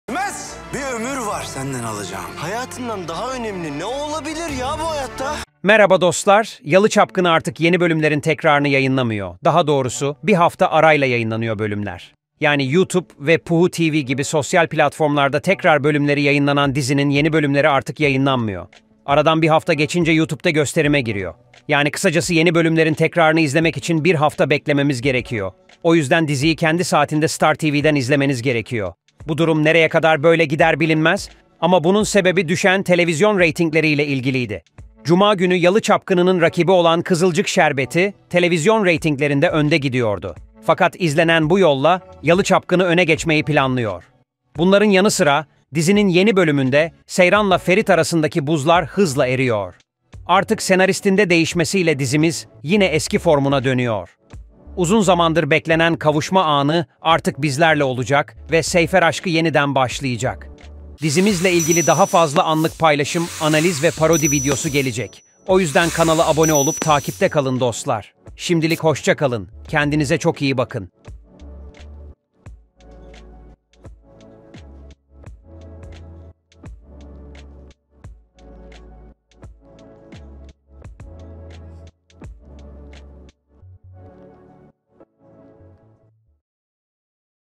Yalı Çapkını 82. Bölüm Analiz
Seyran artık daha fazla kendini geri çekemiyor. Ferit'e olan aşkını itiraf ediyor. Yoksa onu ömrünün sonuna kadar kaybedeceğini biliyor. Artık Seyfer zamanı..

dizinin yeni bölüm tekrarları bir hafta arayla youtubede !